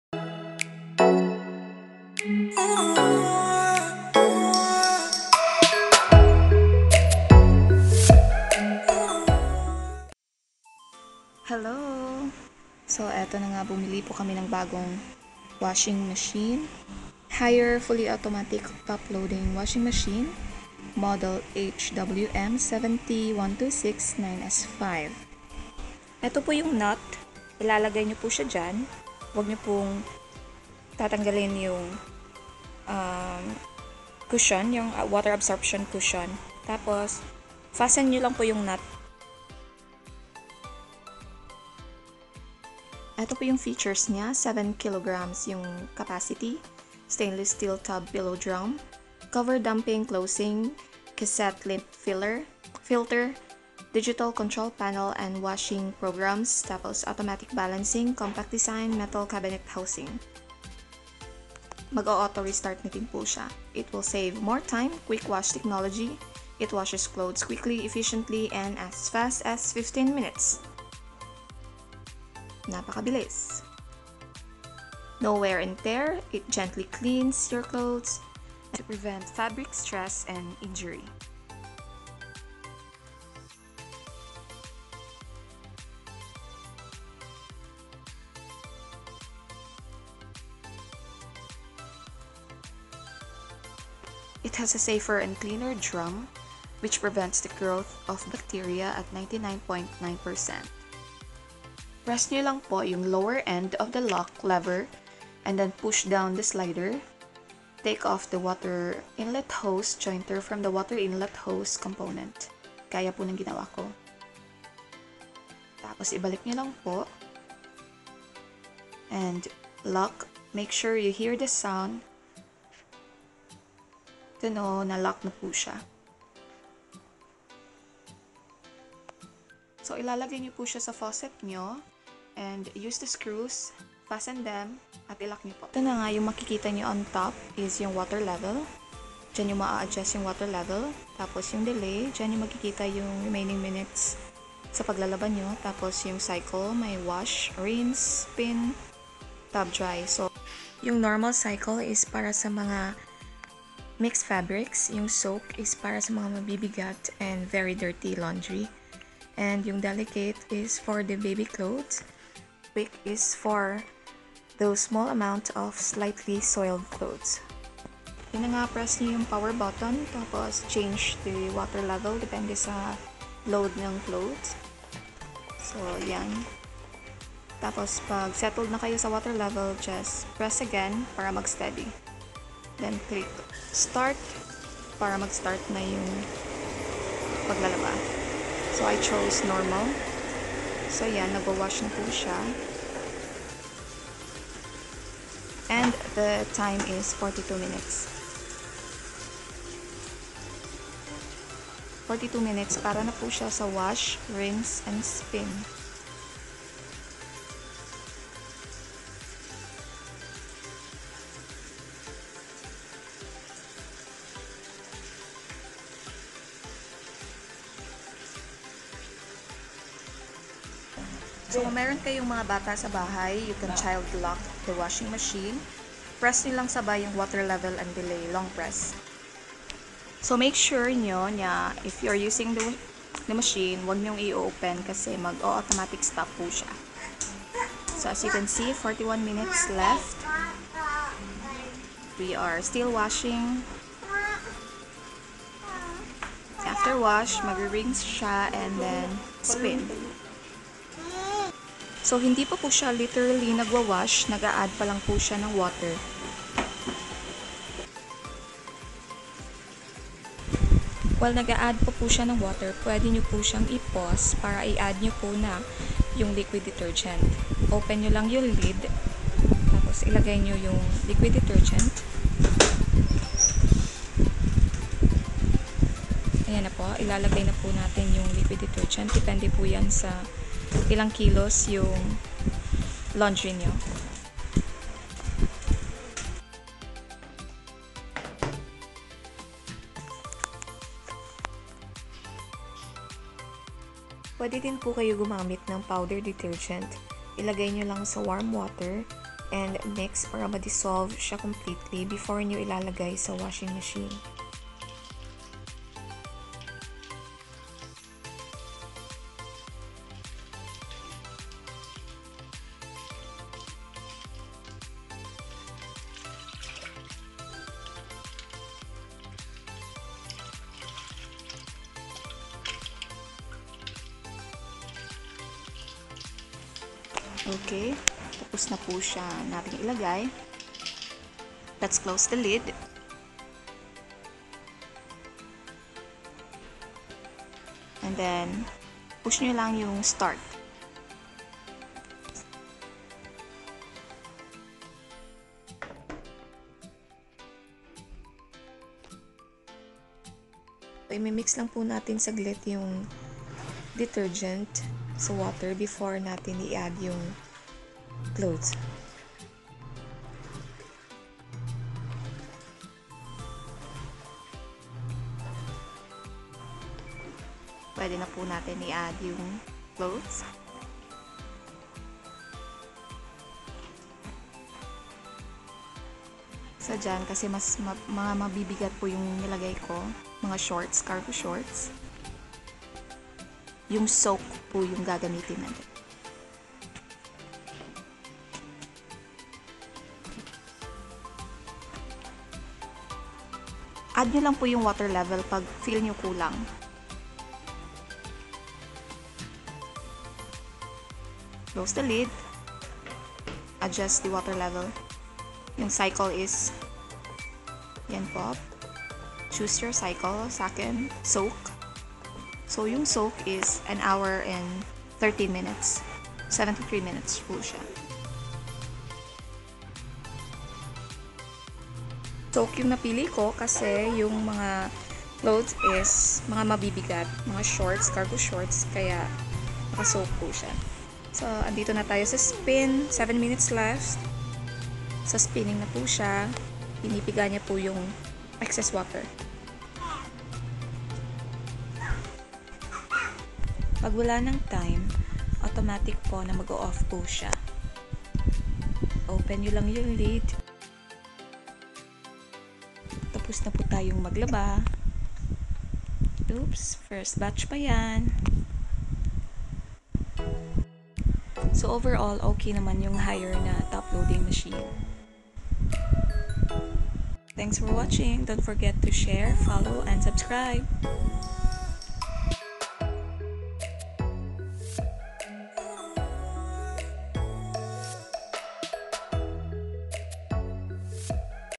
Haier Fully Automatic Top Load Washing Machine Tutorial
Haier Model HWM70-1269S5 Fully Automatic 7kg Top Load Washing Machine. Yay or Nay?